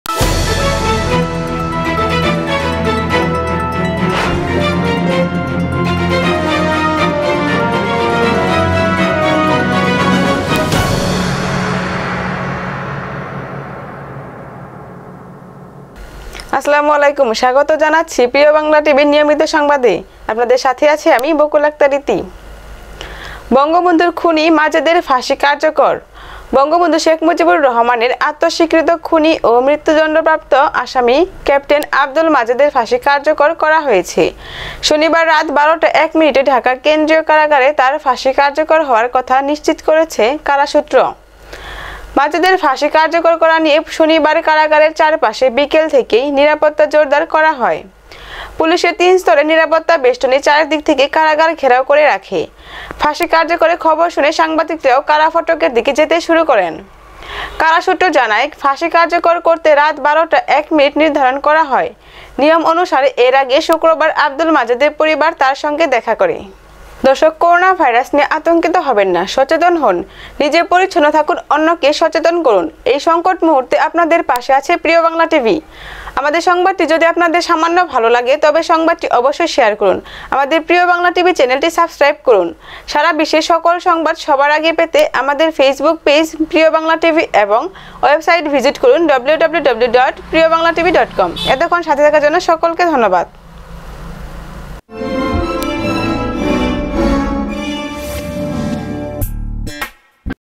বঙ্গবন্ধুর খুনি মাজেদের ফাঁসি কার্যকর priyo bangla tv
priyo bangla tv
“ প্রিয়  বাংলা টিভি”
বঙ্গবন্ধু শেখ মুজিবুর রহমানের আত্মস্বীকৃত খুনি ও মৃত্যুদণ্ডপ্রাপ্ত আসামি ক্যাপ্টেন (বরখাস্ত) আবদুল মাজেদের ফাঁসি কার্যকর করা হয়েছে। শনিবার রাত ১২টা ১ মিনিটে ঢাকা কেন্দ্রীয় কারাগারে তাঁর ফাঁসি কার্যকর হওয়ার কথা নিশ্চিত করেছে কারা সূত্র।

এর আগে ২০১০ সালের ২৭ জানুয়ারি রাতে সৈয়দ ফারুক রহমান, বজলুল হুদা, এ কে এম মহিউদ্দিন আহমেদ, সুলতান শাহরিয়ার রশিদ খান ও মুহিউদ্দিন আহমেদের মৃত্যুদণ্ড কার্যকর করা হয়।

মৃত্যুদণ্ডপ্রাপ্ত পাঁচ খুনি এখনো পালিয়ে আছেন। তাঁদের মধ্যে এস এইচ এম বি নূর চৌধুরী কানাডায় ও এ এম রাশেদ চৌধুরী যুক্তরাষ্ট্রে অবস্থান করছেন। অন্য তিনজন খন্দকার আবদুর রশিদ, শরিফুল হক ডালিম ও মোসলেম উদ্দিনের অবস্থান সম্পর্কে সরকারের কাছে নিশ্চিত কোনো তথ্য নেই। এ ছাড়া ফাঁসির দণ্ডাদেশপ্রাপ্ত আরেক আসামি আজিজ পাশা ২০০২ সালে পলাতক অবস্থায় জিম্বাবুয়েতে মারা যান।

মাজেদের ফাঁসি কার্যকর করা নিয়ে শনিবার কারাগারের চারপাশে বিকেল থেকেই নিরাপত্তা জোরদার করা হয়। পুলিশের তিন স্তরের নিরাপত্তাবেষ্টনী চারদিক থেকে কারাগার ঘেরাও করে রাখে। ফাঁসি কার্যকরের খবর শুনে সাংবাদিকেরাও কারাফটকের দিকে যেতে শুরু করেন।